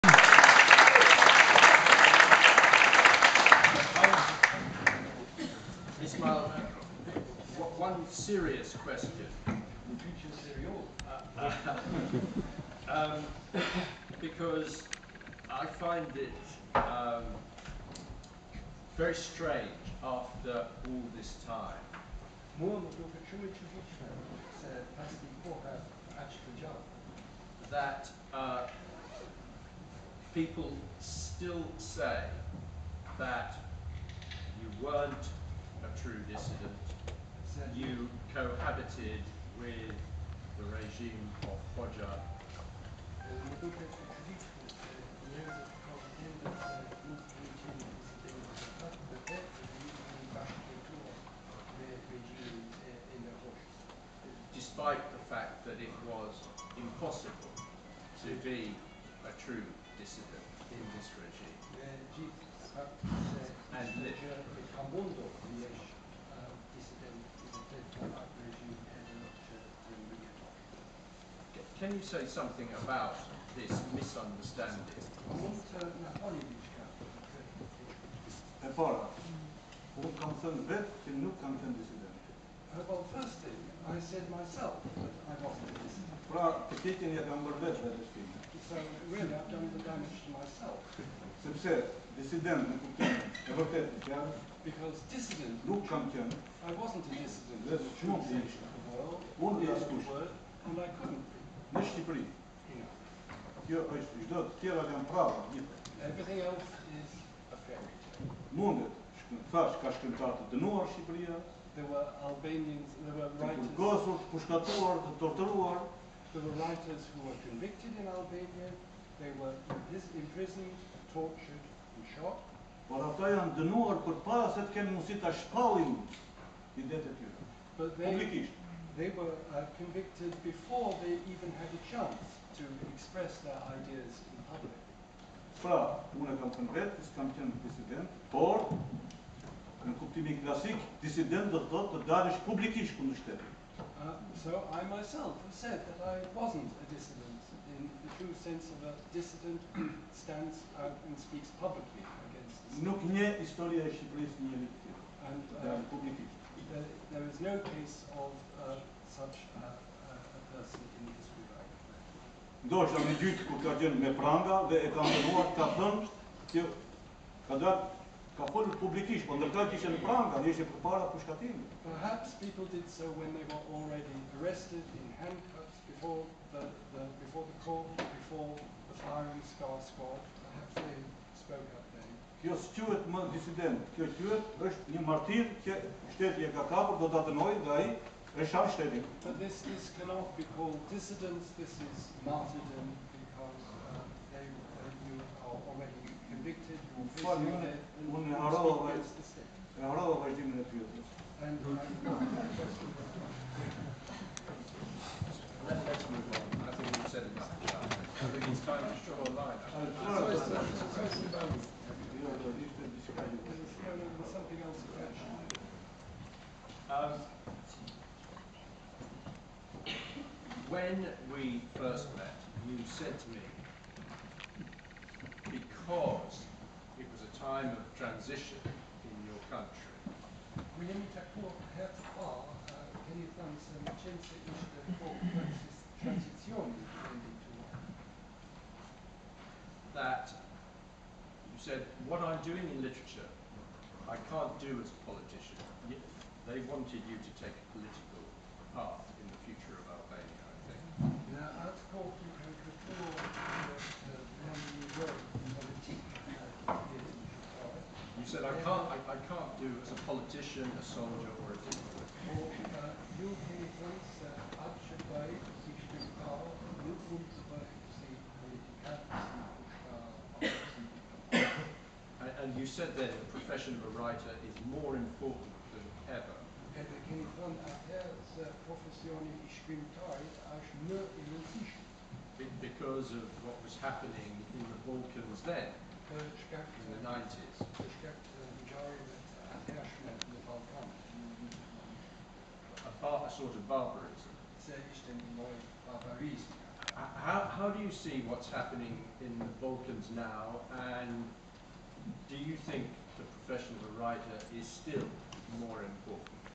Kadare in London, promoting his new novel "The Accident"-- 05 October 2010
The Accident: Ismail Kadare, Tuesday 5 October 2010, 6.30pm, Venue: Free Word Centre, 60 Farringdon Road, London, EC1R 3GA. In association with FLOW - the Free Word Festival.

Born in 1936, Ismail Kadare is Albania's best-known poet and novelist. His international reputation was cemented when, in 2005, he was awarded the first Man Booker International Prize for 'a body of work written by an author who has had a truly global impact.'

His latest novel, The Accident, intimately documents an affair between two people and their mutually destructive obsession that mirrors the conflicts of the region. In a rare UK appearance, Kadare discusses his work with the author and chair of English PEN's Writers in Translation committee, Julian Evans.